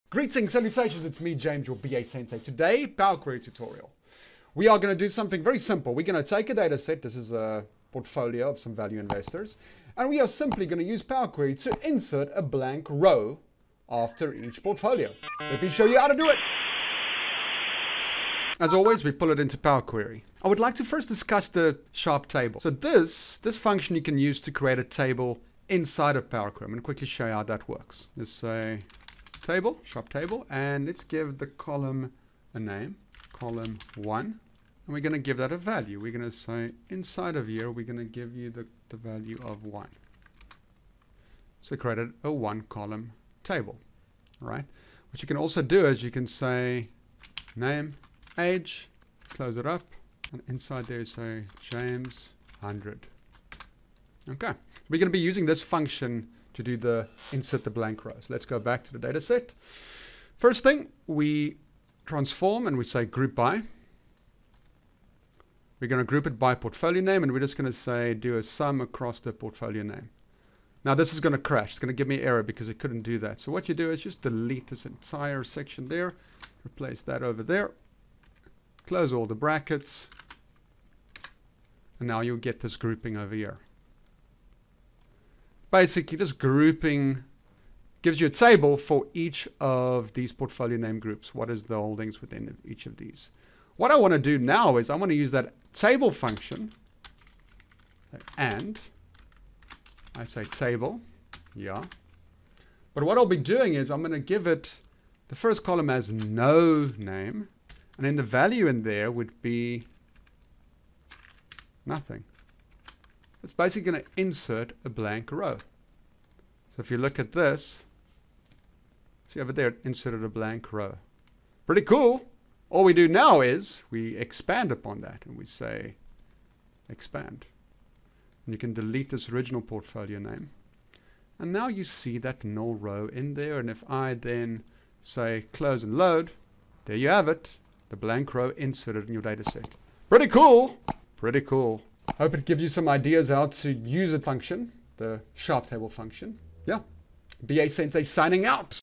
Power Query - Add Blank Row After Grouping
In This video i show you how to add a blank row after each grouping in a dataset using the table function.

Links